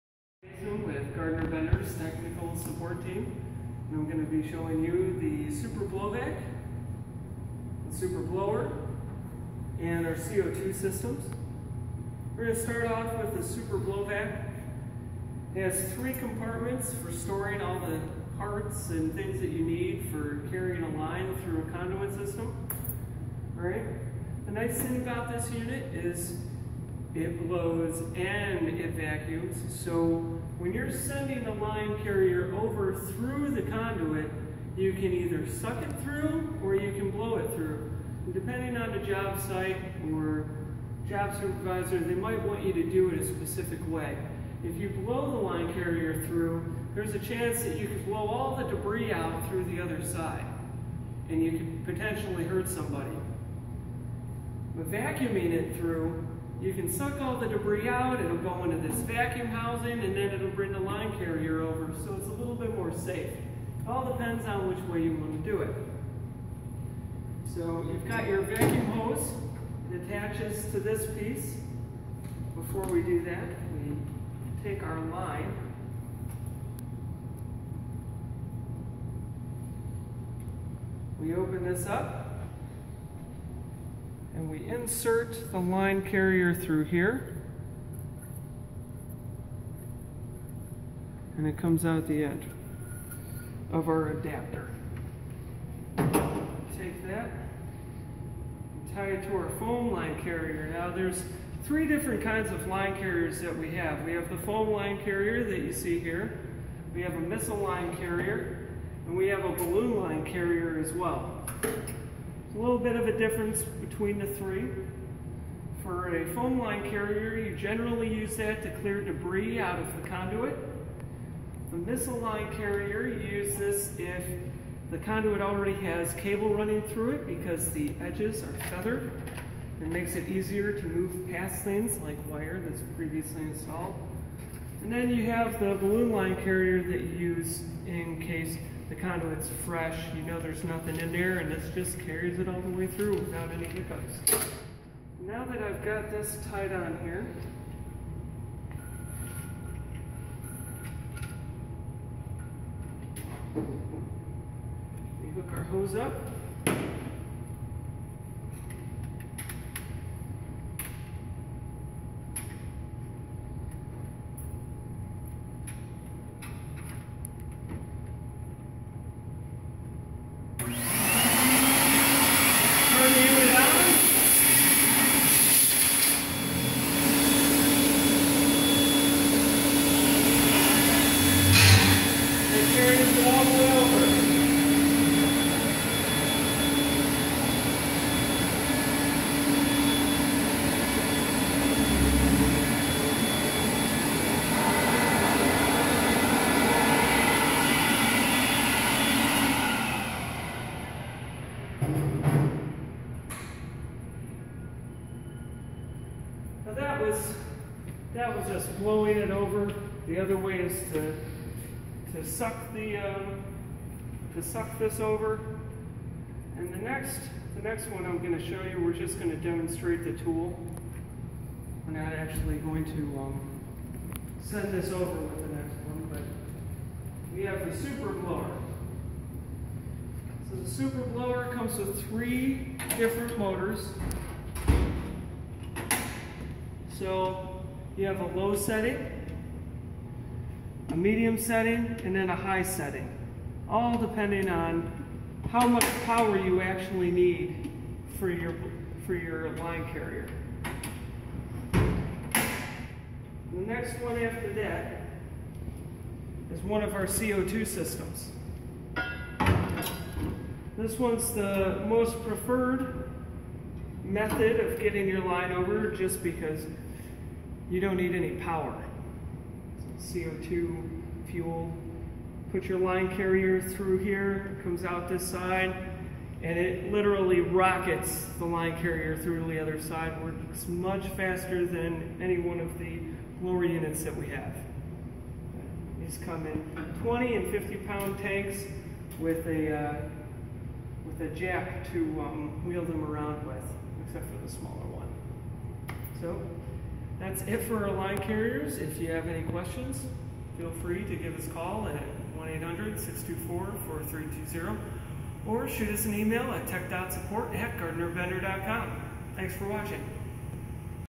Line Carrier Systems - Gardner Bender
Instructions on how to use our line carrier systems. The B60, BV208DA, and the CO2 Power House Conduit Fishing Systems JO225LC and the Powr-Pak JO25..